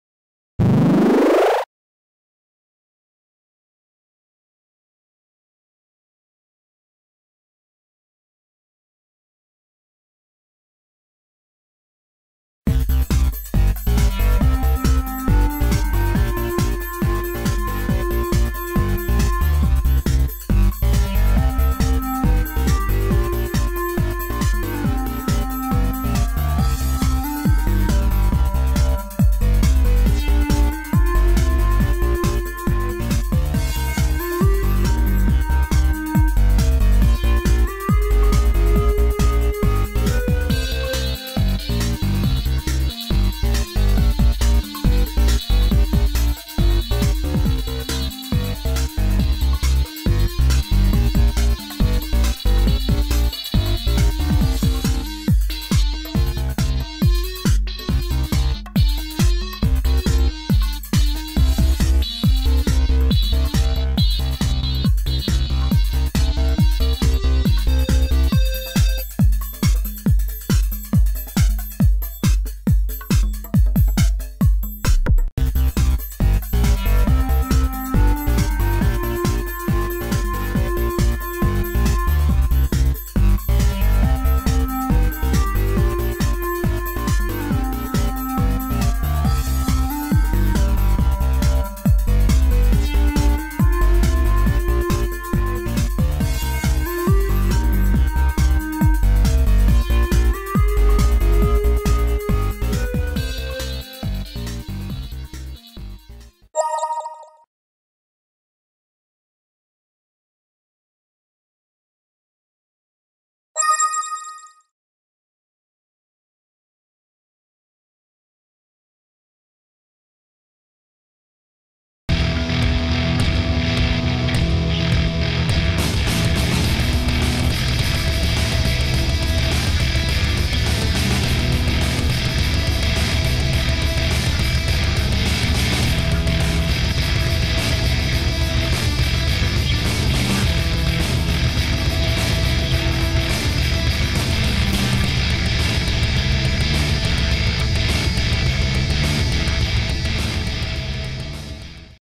Minecraft Art - Sniper X Loner
Minecraft Art I made for SniperXLoner! I was also eating some yummy oatmeal cookies during the coloring process. :3 Enjoy! ^_^